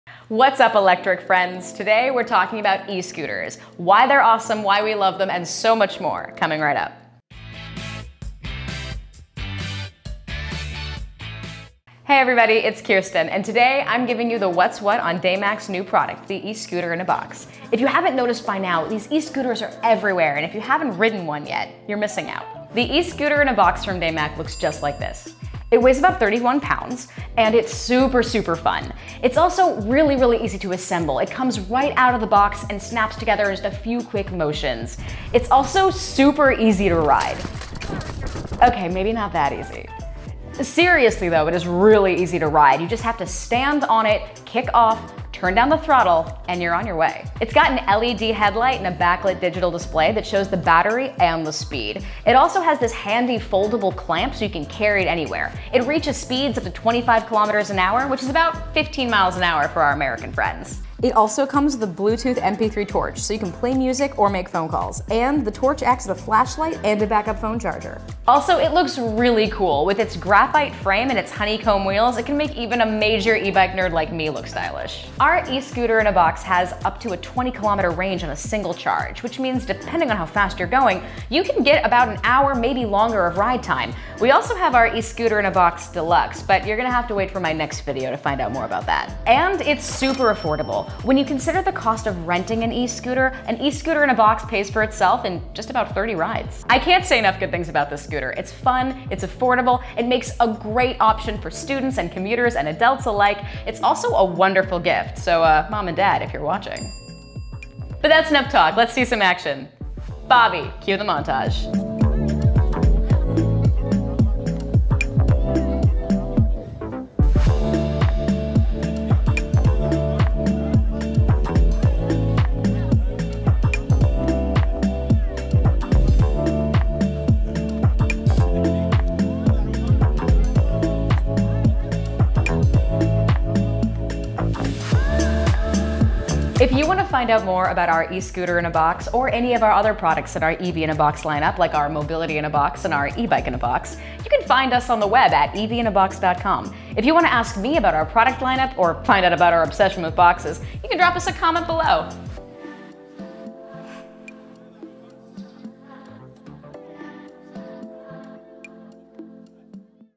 Escooterinabox | Foldable Electric Scooter by Daymak
Electric scooters are awesome! Faster than a conventional scooter, with a high longevity and excellent performance, Escooters are perfect for short commutes, travel, and leisure. Not to mention they make wonderful gifts!

Our new Escooterinabox from our EVinabox series assembles in just a few snaps. But it has so many more amazing features we know you’ll love. Let’s take a look!